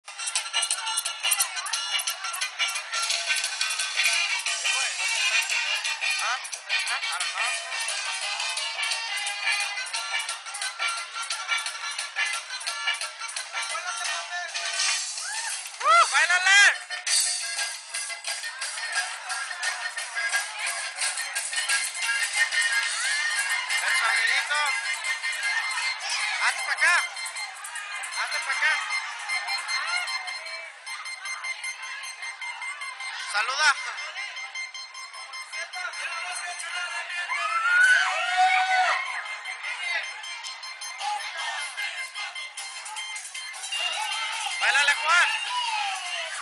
dancin at lunch at armijo
dancing after the performance at lunch